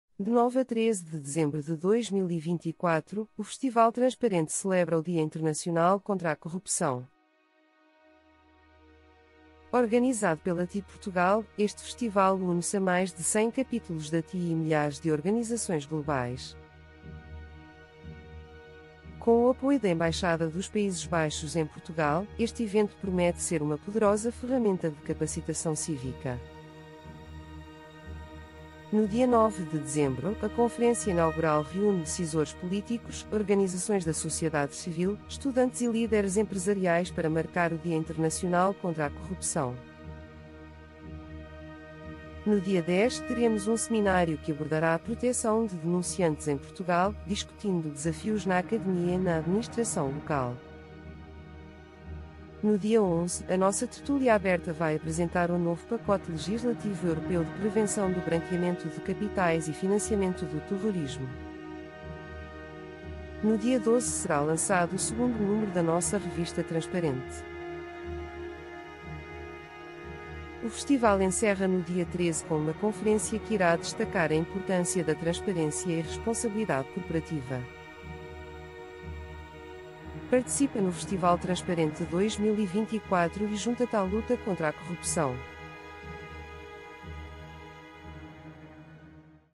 Festival Transparente 2024
A 9 de dezembro celebra-se o Dia Internacional contra a Corrupção, assinalando a 21º aniversário da Convenção das Nações Unidas (UCAN). Como habitualmente, a TI Portugal organiza – de 9 a 13 de dezembro de 2024 – uma nova edição do Festival Transparente, juntando-se assim aos mais de 100 capítulos da Transparency International, coligação mundial de ONG anticorrupção, e aos milhares de organizações e movimentos da sociedade civil espalhados pelo mundo, na celebração do combate à corrupção como instrumento de capacitação cívica e de proteção dos Direitos Humanos.
O Festival Transparente 2024 tem o apoio da Embaixada dos Países Baixos em Portugal.
Sabe mais em transparencia.pt/festival-transparente-2024